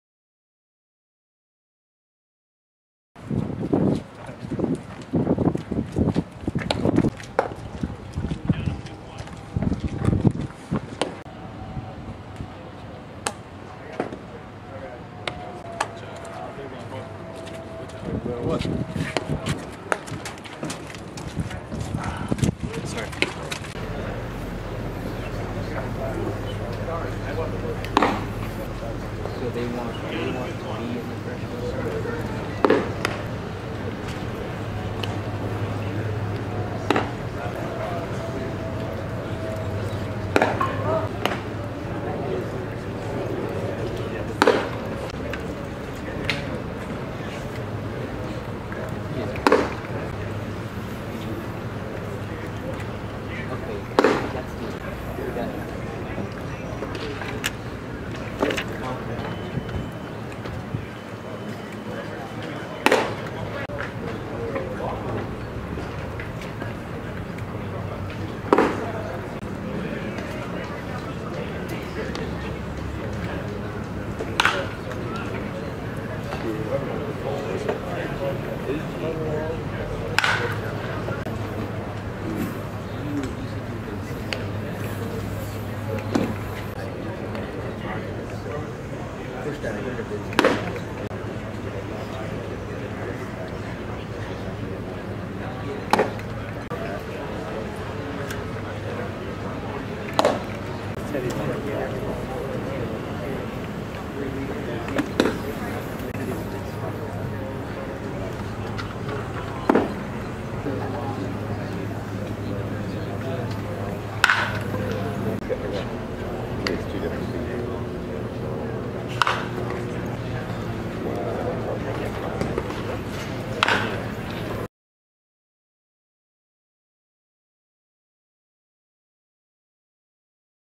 Tyler Mahle Prospect Video, Westminster High School
Tyler Mahle of Westminster High School pitches at the Area Code Games tryouts at USC